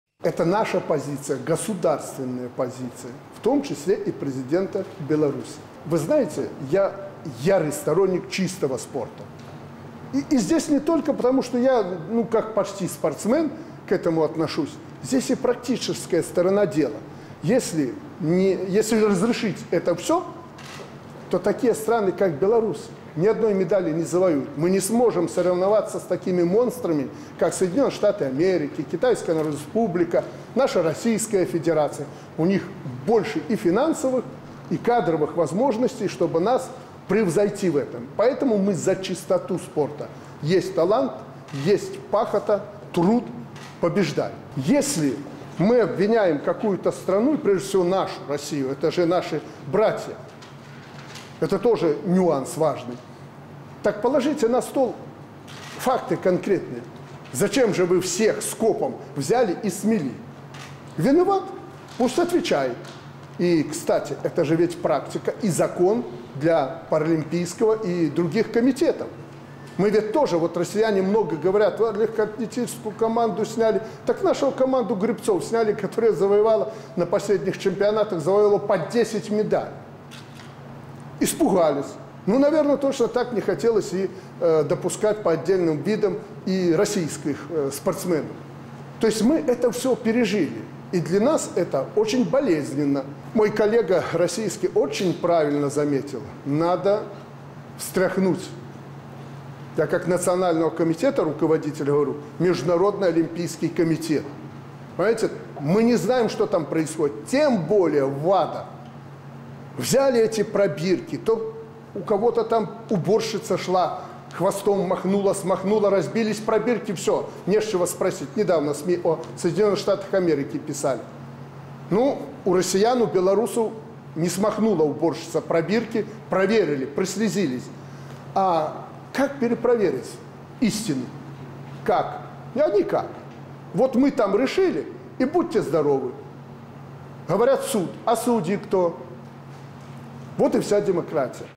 Александр Лукашенко прокомментировал вынос белорусами российского флага на Паралимпиаде
Александр Лукашенко проголосовал на избирательном участке №509 Старовиленского избирательного округа №105, который располагается в Белорусском государственном университете физической культуры.

Глава государства поприветствовал наблюдателей, в том числе и иностранных, а затем пообщался с прессой.